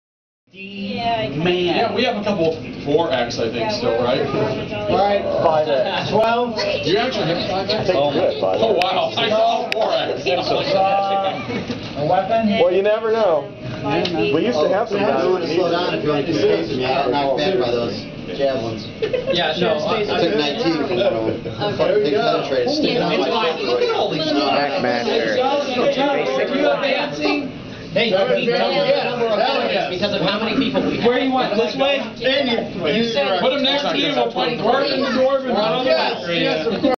Hackmaster at Origins 2010
A very large group gathered for Hackmaster in the Kenzerco Suite in Columbus at Origins 2010.